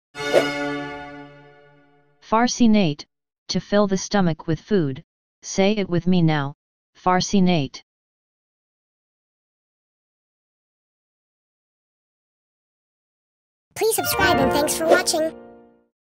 farcinate - Smart & Obscure English Words Defined 🗣🔊
This video is about the word "farcinate". Here's a video of some of the most obscure English words with definitions that will definitely make you sound smart (more smarter) if used properly.